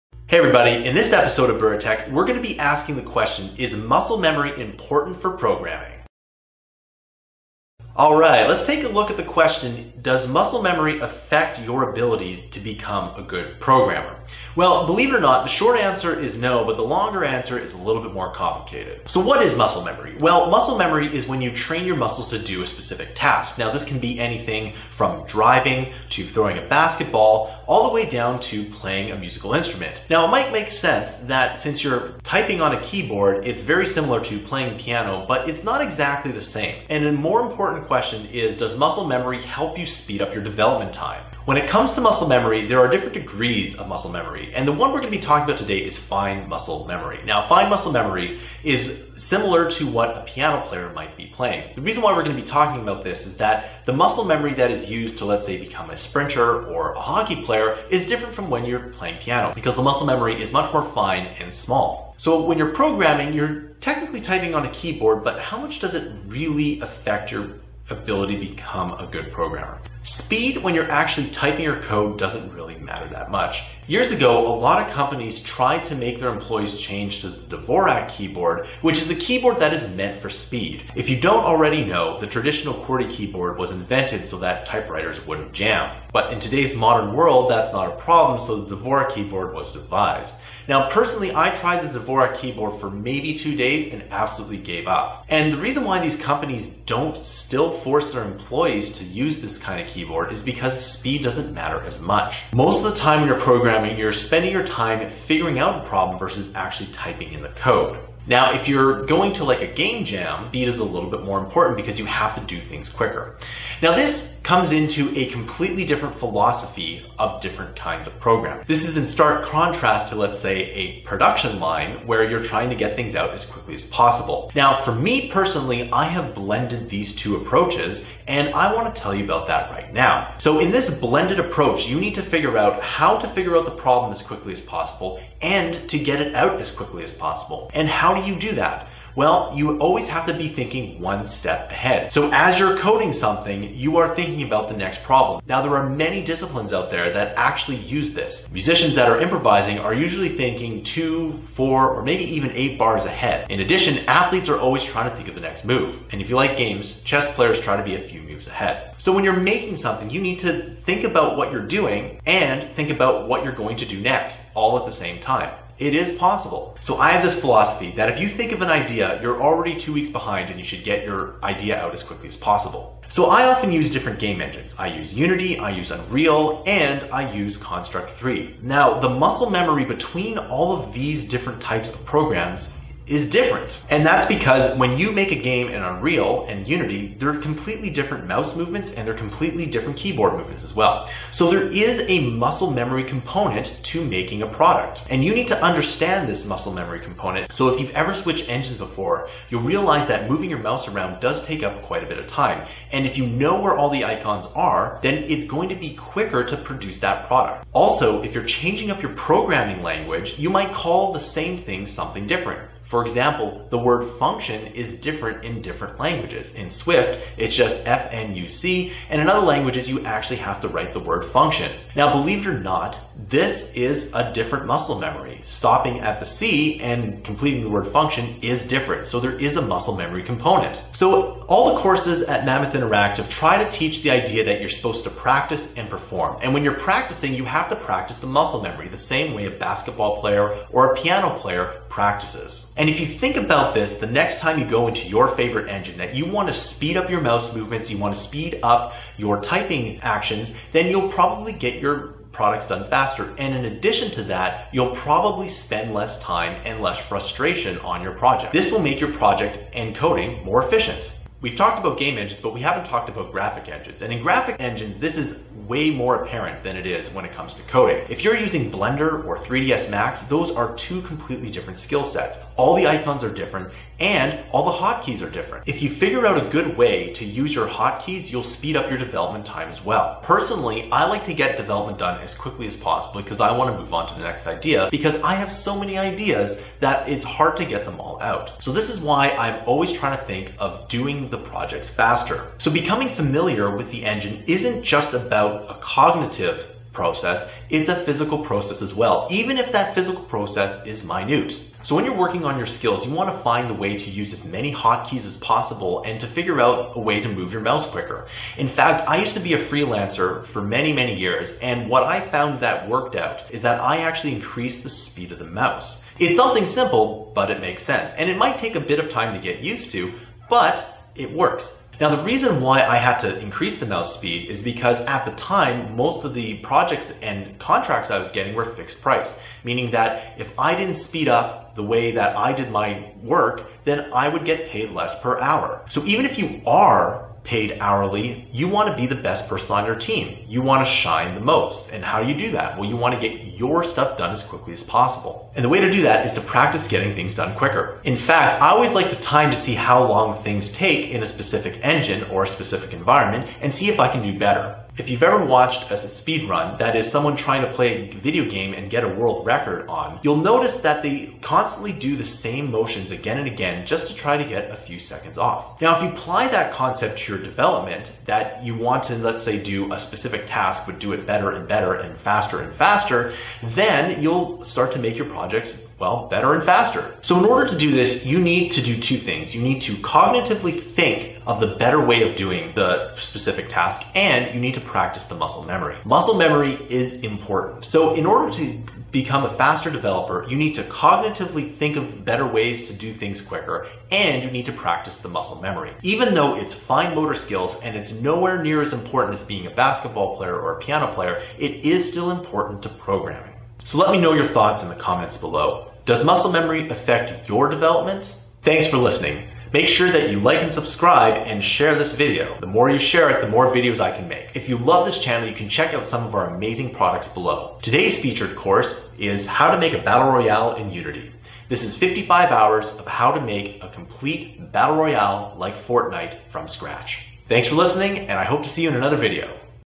Is muscle memory important for programming?
What is muscle memory, and how does it play a role in coding? Let's discuss the "muscle memory" effect and its importance in software development.

-John Bura